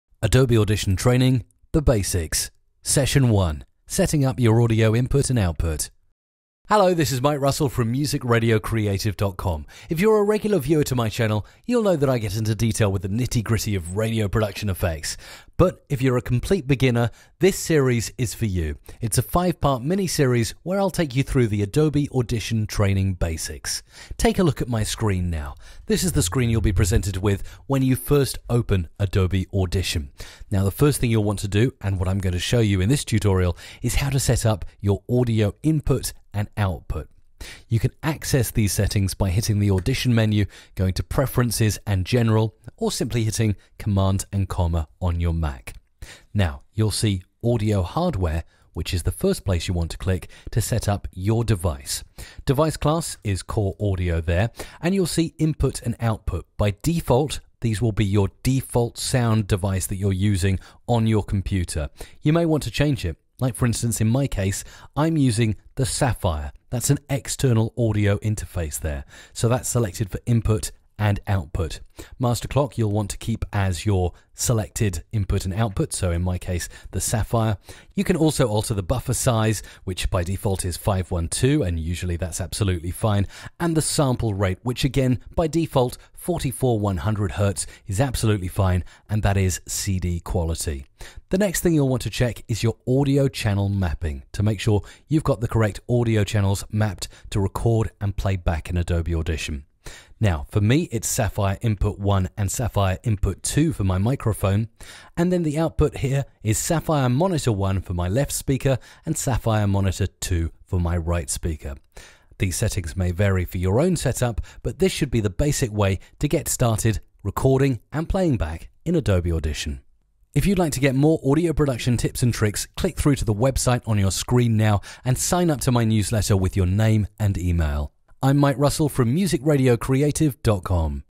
Setting Up Audio Input & Output - Adobe Audition Training
Adobe Audition Training (The Basics) : Here's how to set up your audio input and output to work with Adobe Audition. This is the first step you'll need to take before starting to record and edit in Adobe Audition.